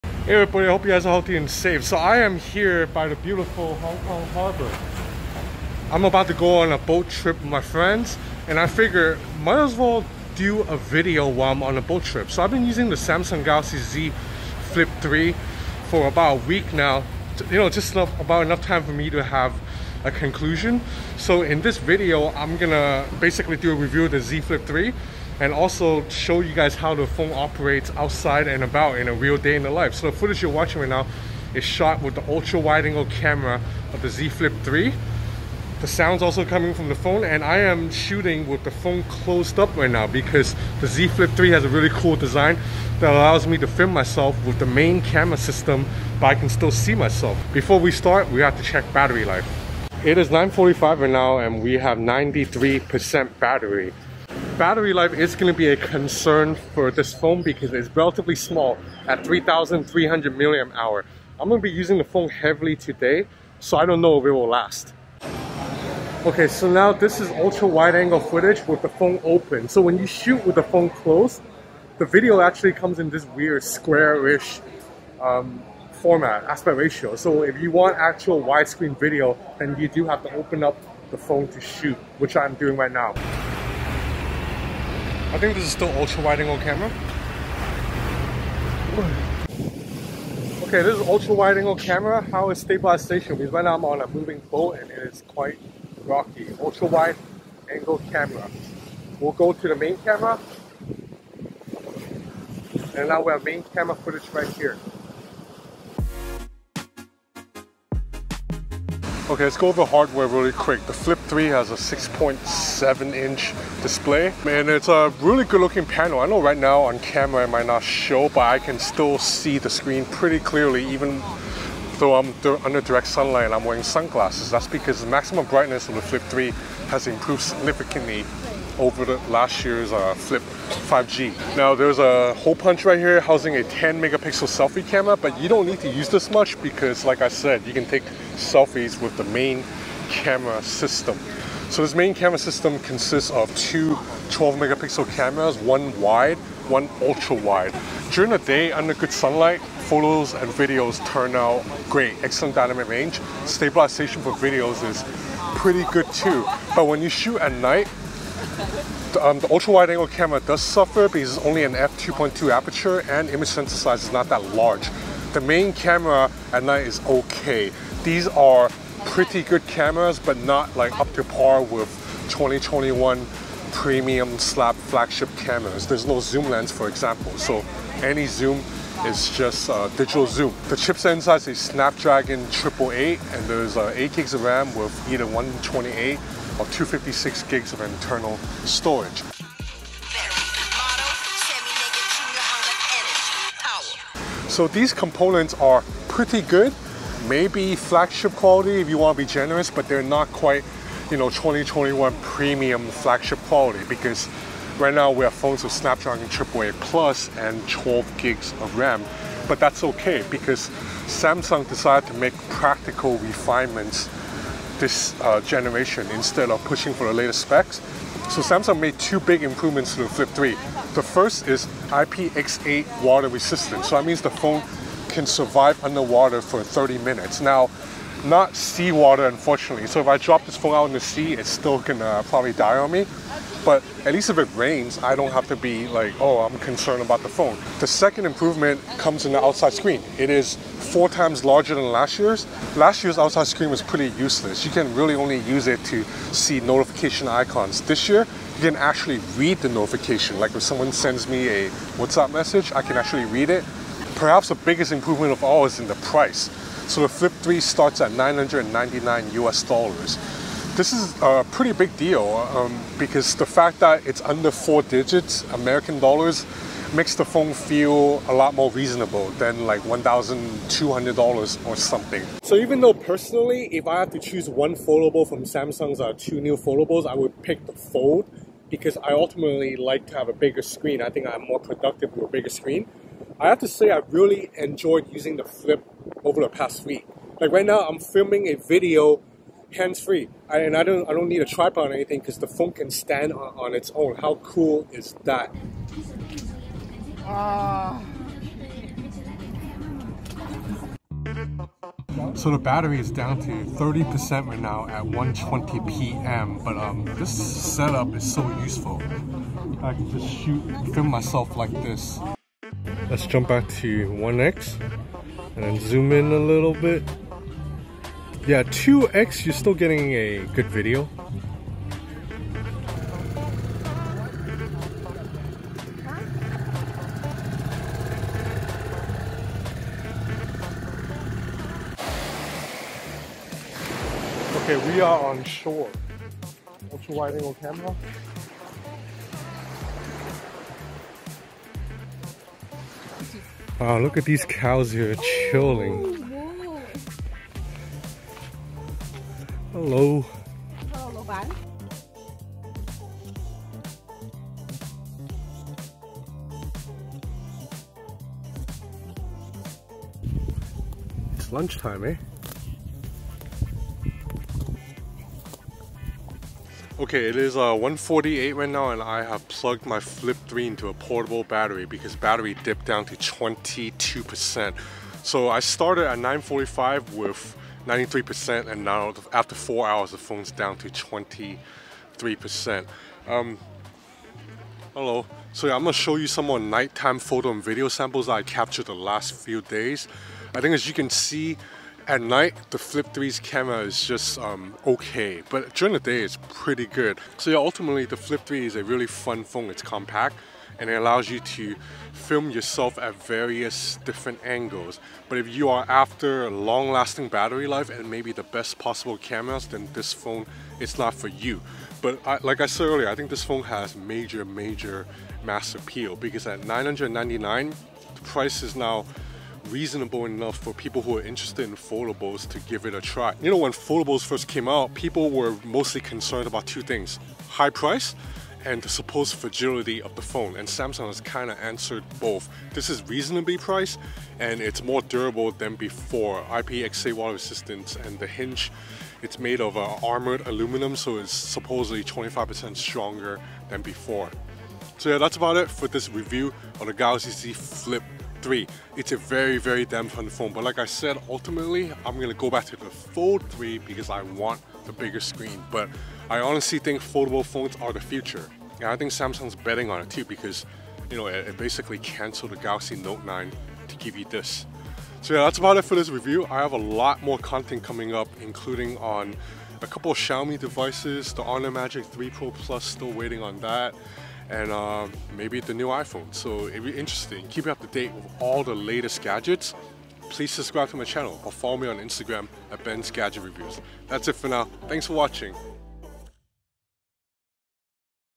Galaxy Z Flip 3 Real-World Review -- On A Boat
I've been using the Samsung Galaxy Z Flip 3 for a week, and I decided to shoot the review while on a day out around Hong Kong on a boat.

TIME CODES:
0:00 - Flip 3 ultra-wide footagem, with phone closed
1:15 - Flip 3 ultra-wide footage
1:40 - Flip 3 hardware and specs
3:09 - Flip 3 speaker test
4:33 - Flip 3 selfie camera footage
5:42 - Flip 3 video samples
6:55 - Flip 3 final thoughts and conclusion